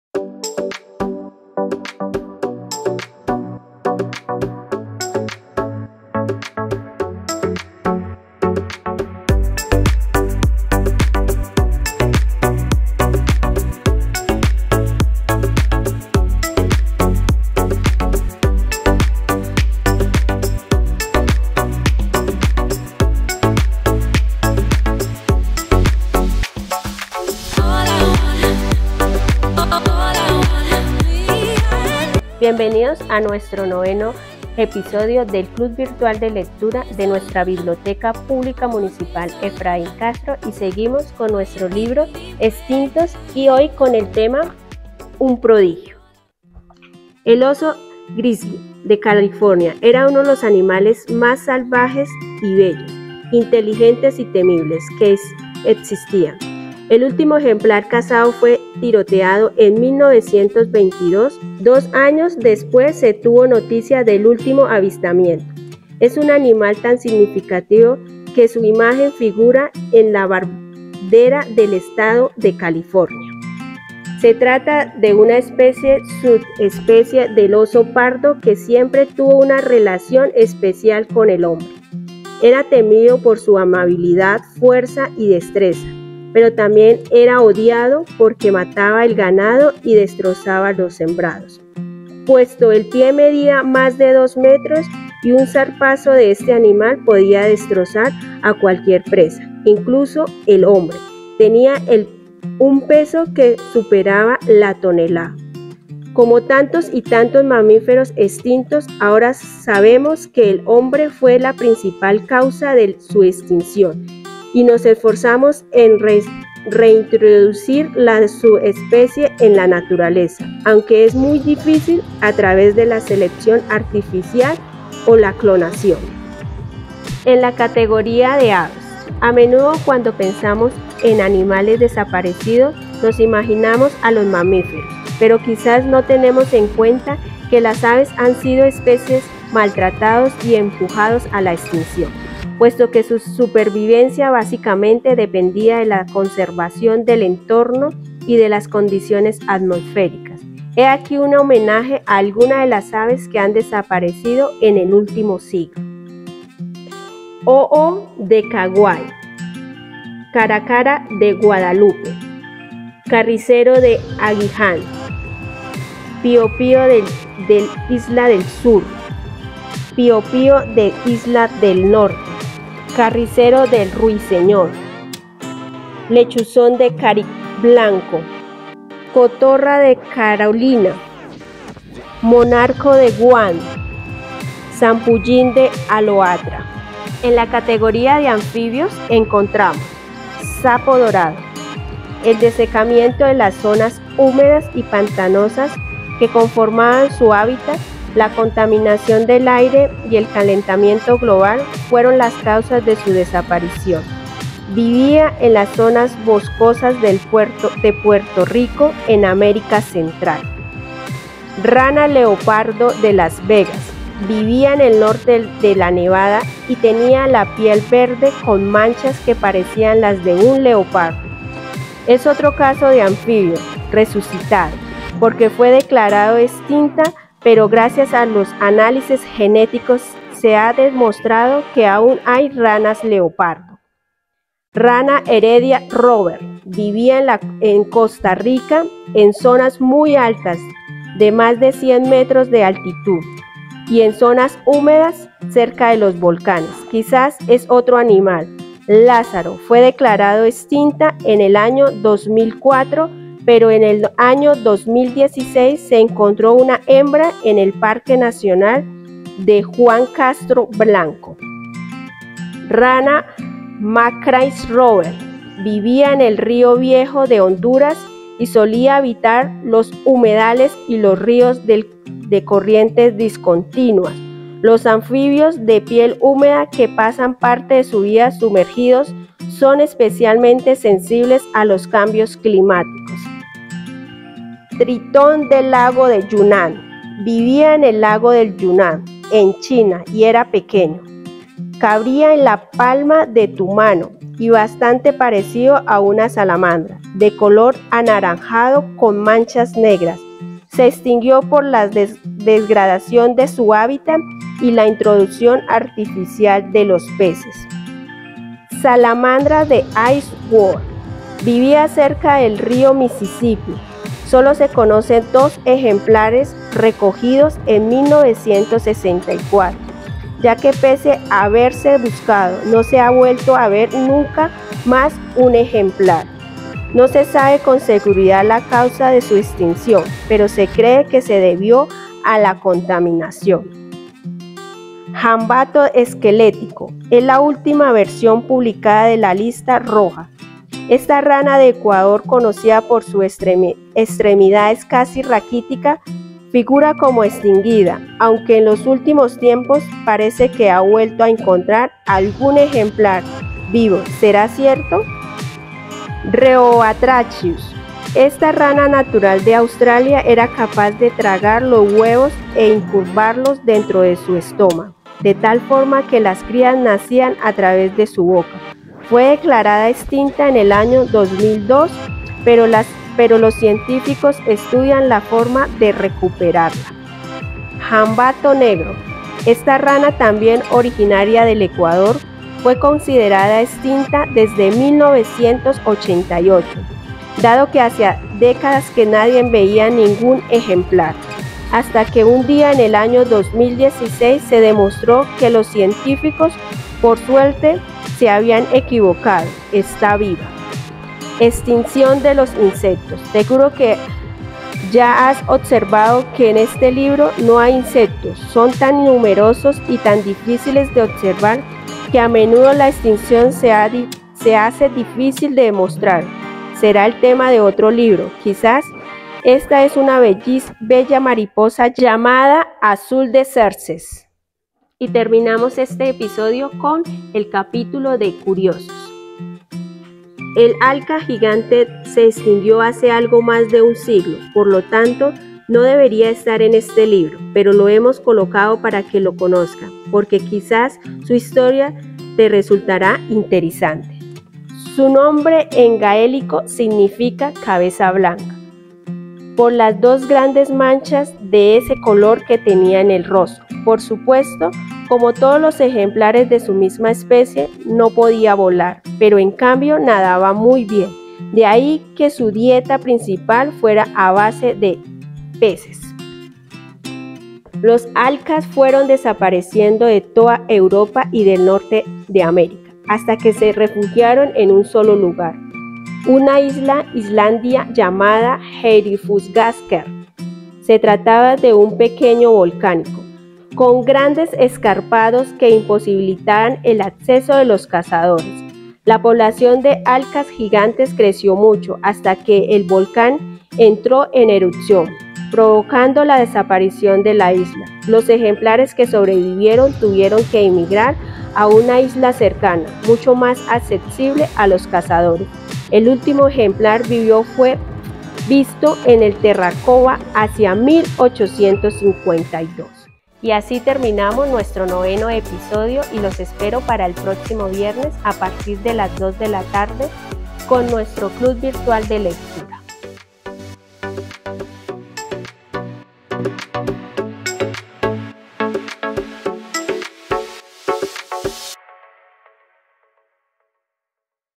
Club Virtual de Lectura | Biblioteca Pública. Ep. 9
✨En este noveno episodio de nuestro Club Virtual de Lectura de la Biblioteca Pública Municipal "Efraín Castro", hablaremos de un animal muy tierno que lamentablemente ha desaparecido por la caza humana.

✨Conoce especies que han desaparecido de la tierra y aprende a cuidar del medio ambiente en el que múltiples especies coexistimos.